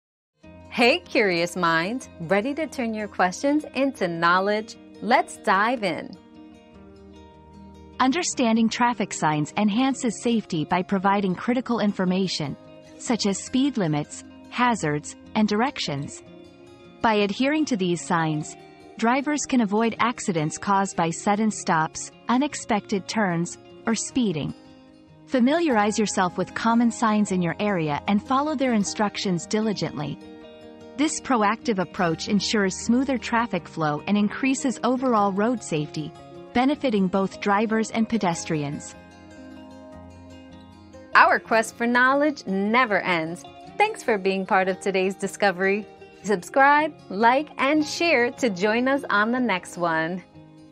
How can understanding traffic signs prevent accidents?
Decoding Traffic Signs: Key to Preventing Accidents 👉 Decoding Traffic Signs 👉 Learn how understanding traffic signs can prevent accidents by providing crucial information on speed limits, hazards, and directions. Following these signs diligently leads to smoother traffic flow and increased road safety for drivers and pedestrians alike.

Laura S. Harris (2024, June 27.) How can understanding traffic signs prevent accidents?
    🌐 AskAbout.video